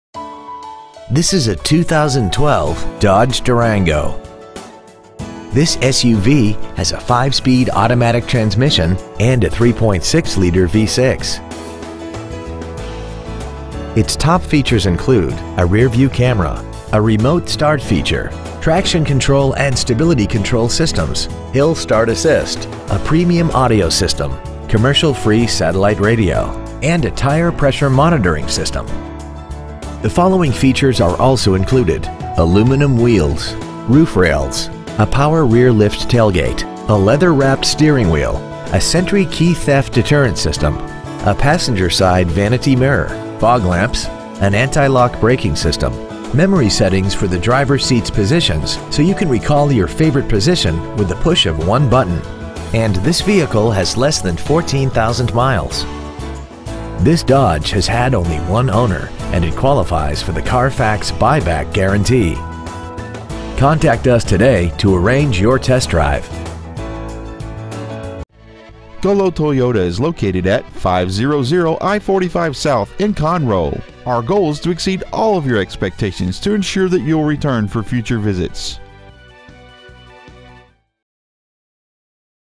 2012 Dodge Durango Tomball TX
Year : 2012
 Make : Dodge
 Model : Durango
 Engine : Gas/Ethanol V6 3.6L/220
 Trans . : Automatic
 Exterior : Black Clear Coat
 Miles : 13,390
 Interior : Dark Graystone/Medium Graystone Interior
 Stock : X21595B

 2012 Dodge Durango Conroe Texas